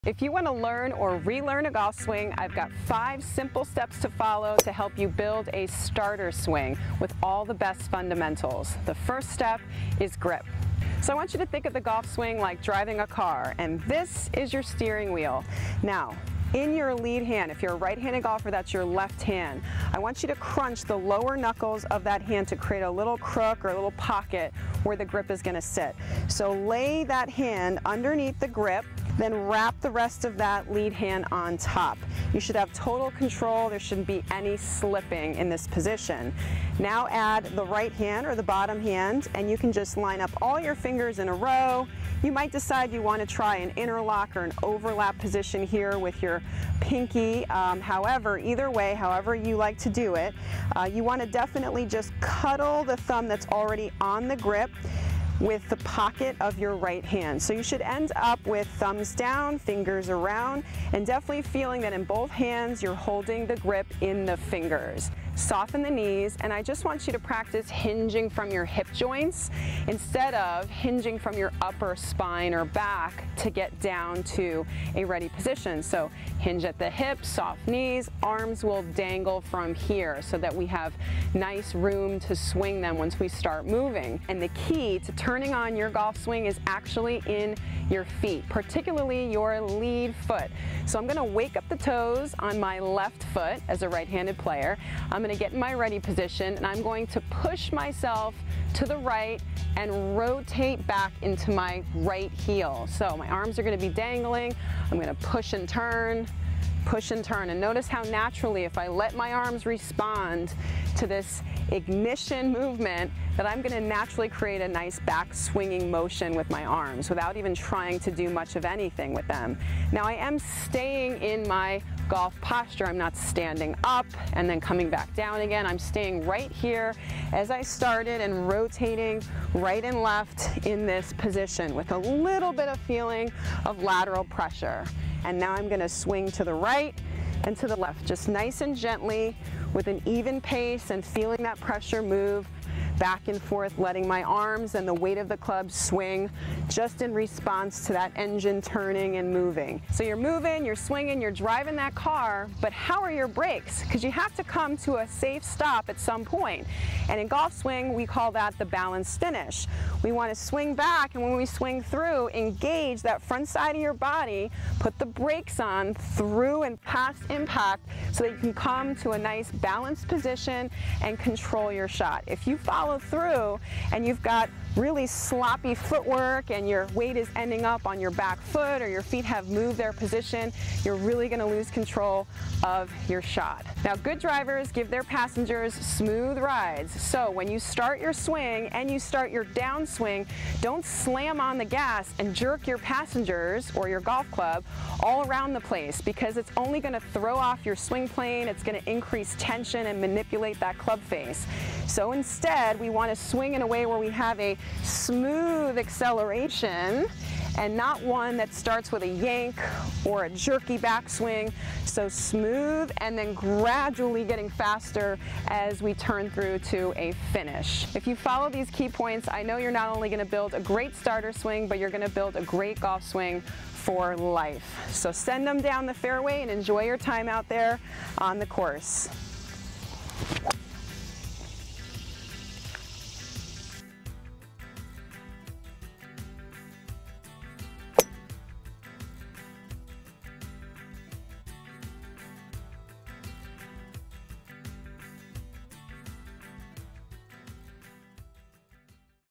How to swing a golf club: 5 steps for beginners
The golf swing is a complicated movement, but it doesn't need to be! We explain the basics of building your swing from scratch.